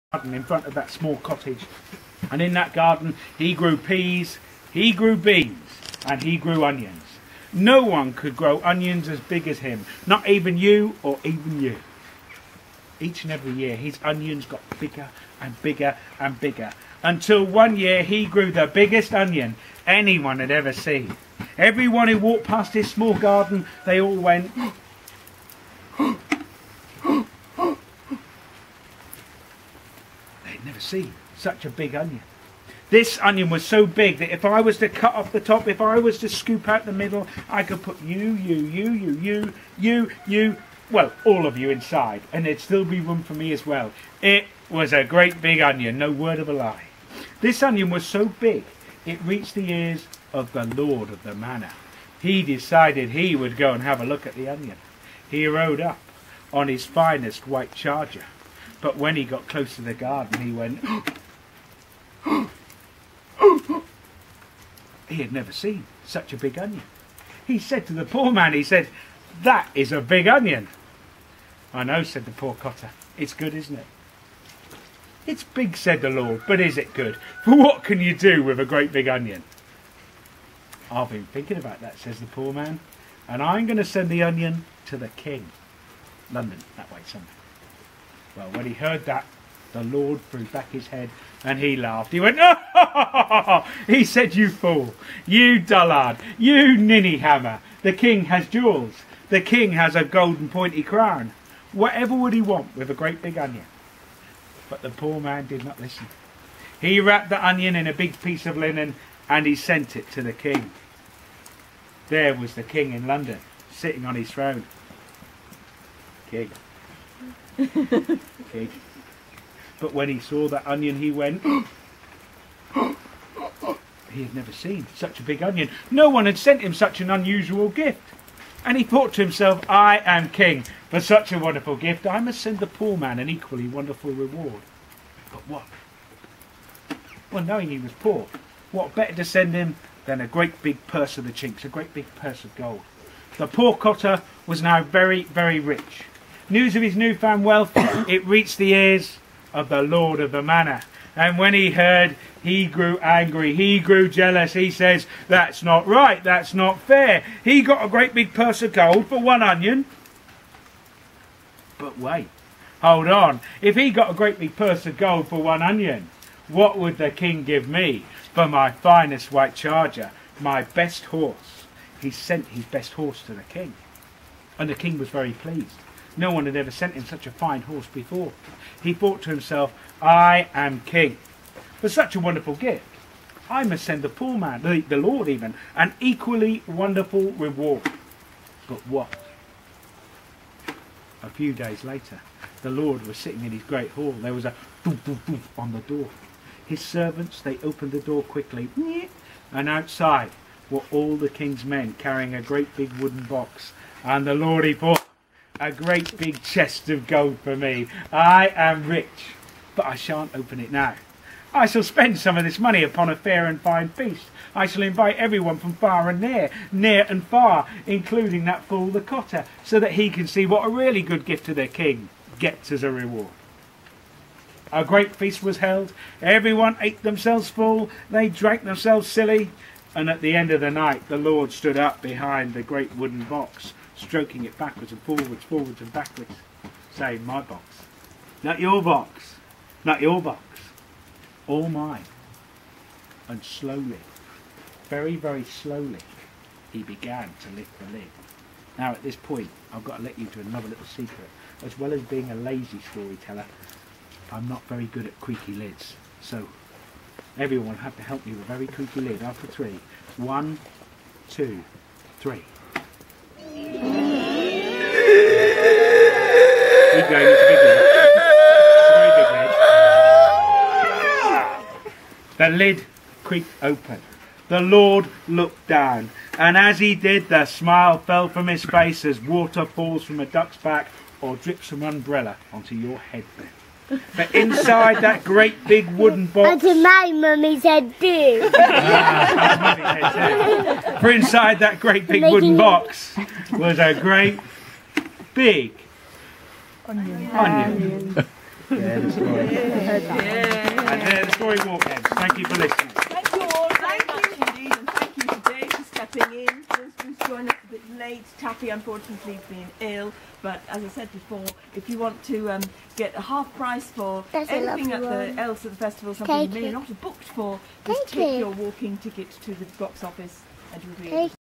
Settle Storytelling Festival and Dave Tong
"There was once a cotter who had a garden where he grew wonderful vegetables. " Dave Tong tells this story of the crofter who grew an enormous onion, and what the Lord of the Manor and the King thought about it. This was on the Storytelling Walk up to the allotments in Settle in the Yorkshire Dales, in the rain.